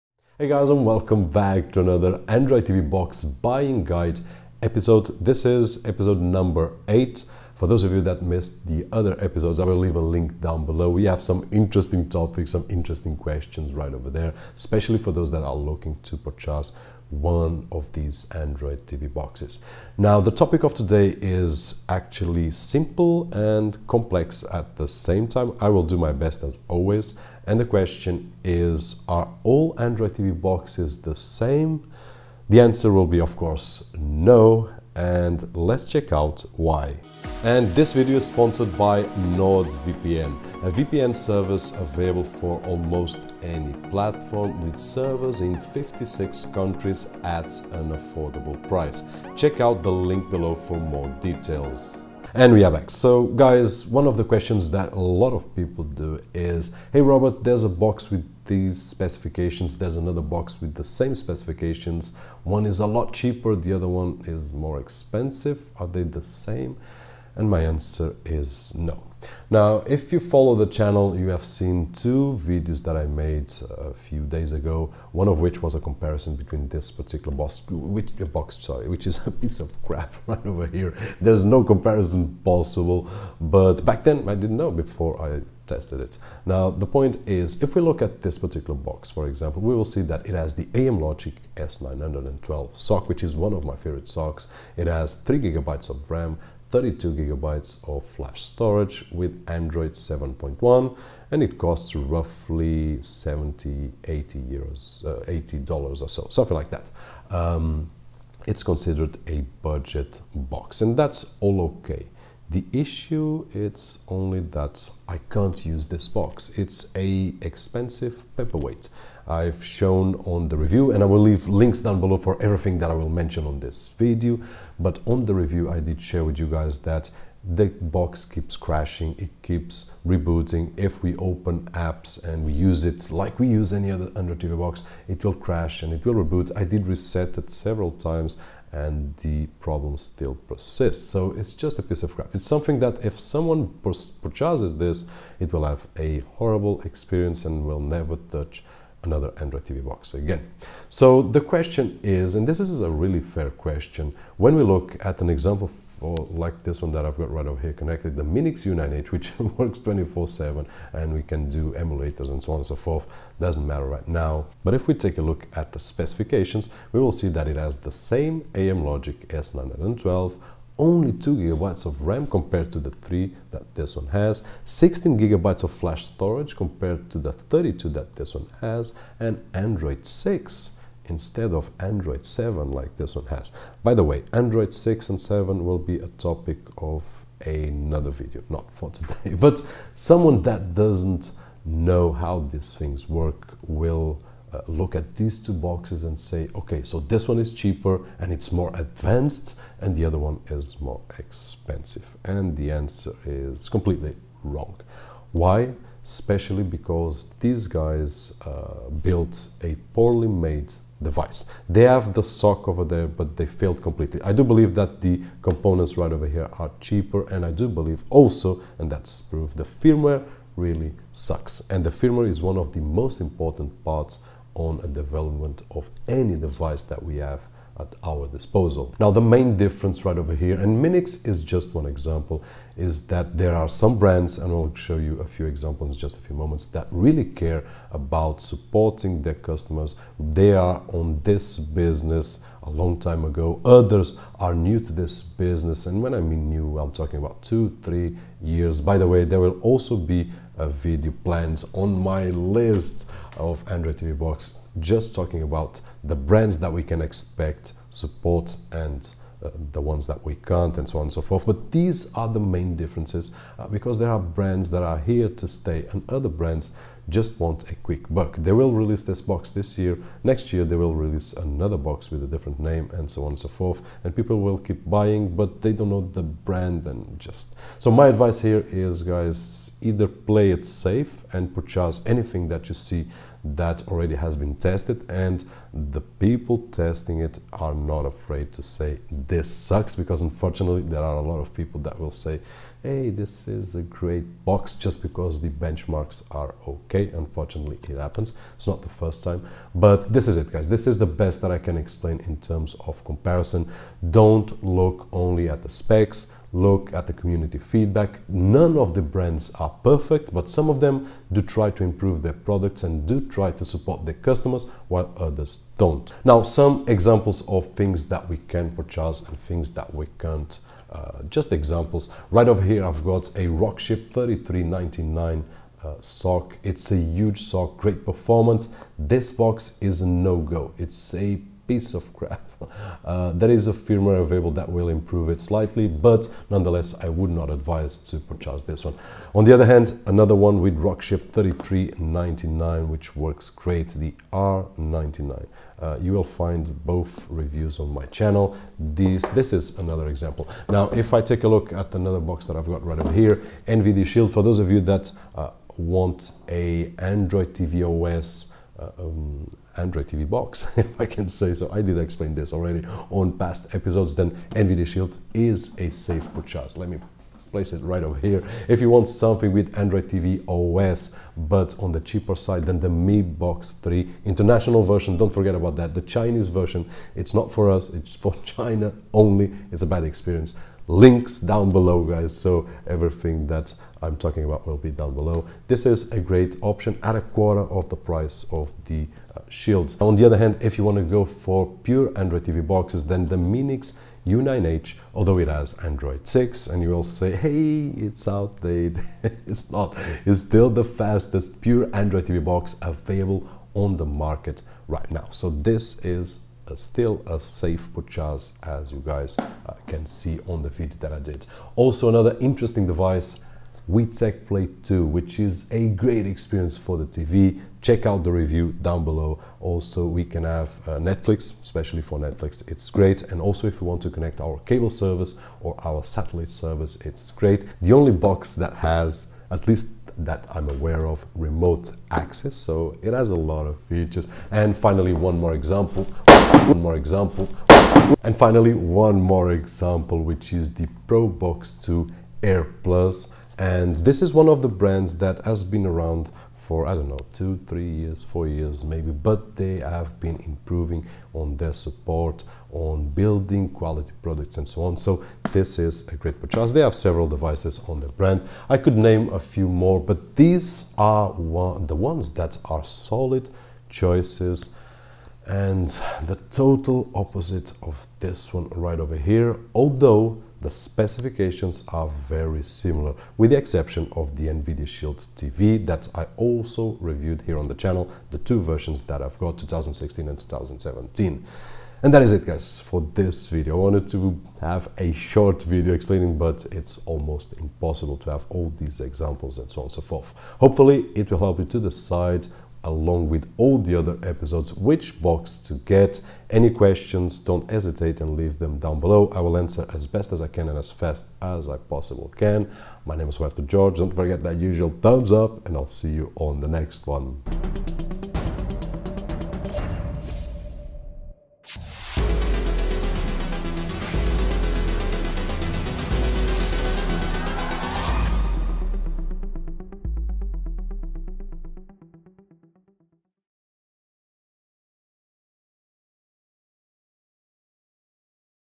ARE ALL ANDROID TV BOXES THE SAME ?
Episode 8: ARE ALL ANDROID TV BOXES THE SAME ?

In this series of videos we will try to help you to decide which is the Best Android TV Box for each one of us.

✅ SUGGESTED VIDEOS  ✅

✅ RECORDING GEAR: ✅